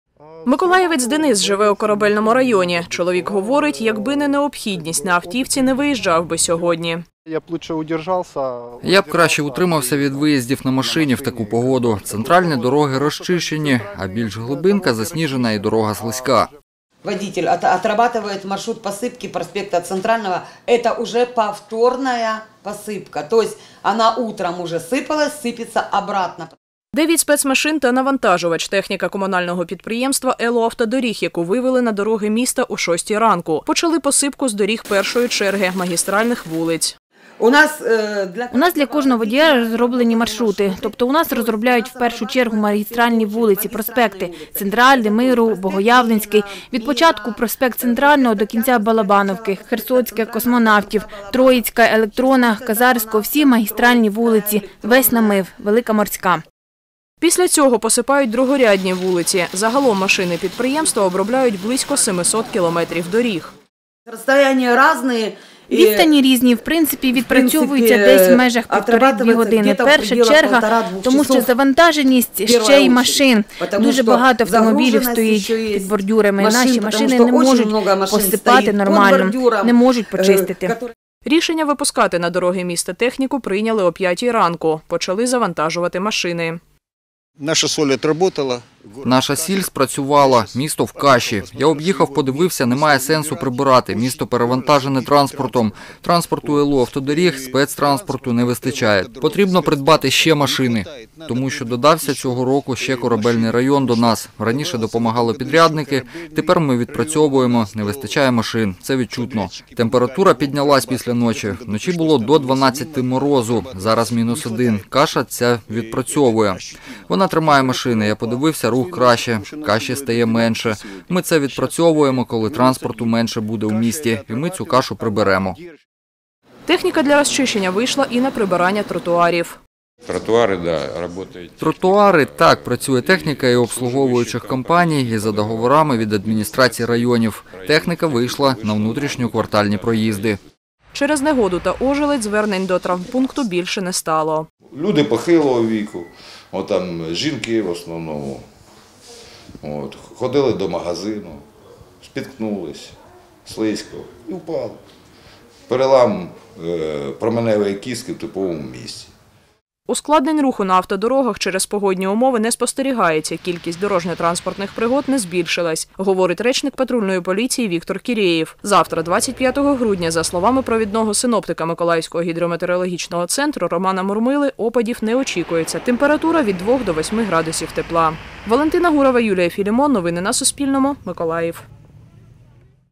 Снігопад у Миколаєві. Як працювали комунальники
Близько 110 тонн реагентів висипані на дороги Миколаєва комунальними підприємствами. За інформацією патрульної поліції ситуація на дорогах спокійна. До травмпункту з травмами легкого ступеня, отримані через погодні умови, звернулось двоє людей.